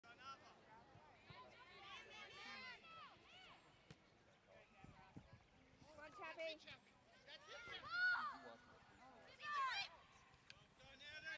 Game 1 vs Lady Chiefs Premier 01 (Atlanta Fire Select Elite Cup) 001 3 clip
Deborah settles the ball in the midfield and quickly sends it up to Chappie who tries to take a shot from distance, but it's deflected by the defender.

Under 16 Girls Gold
Lady Chiefs Premier 01 vs Liberty SC East 01 Red
Scott Hudgens Park, Field 5, 12:15PM